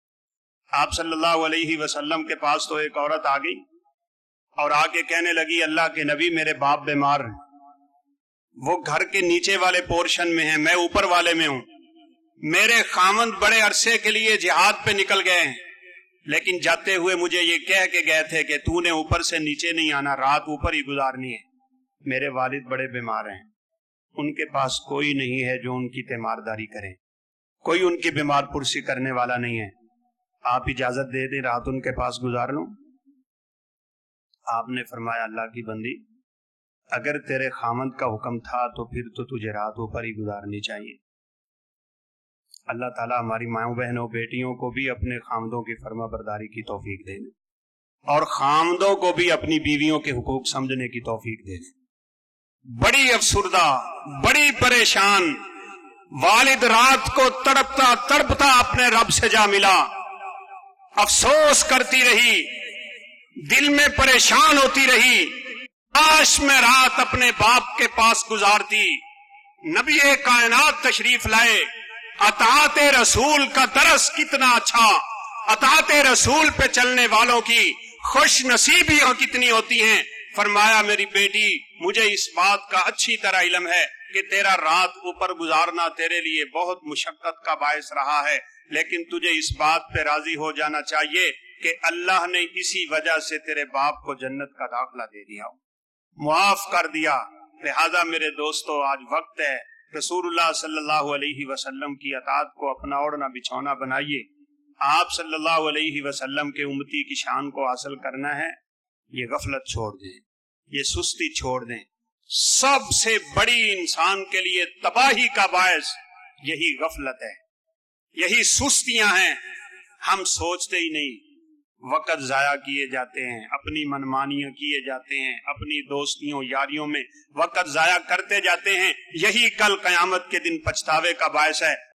Ettat E Rasol ﷺ pr Chalny Wali Aik Sahabia Ka waqia || Shaikh Hafiz Ahmed Siddique Sahib || New Clip
دارالفلاح کے تعلیمی منصوبے:
 دارالفلاح لائبریری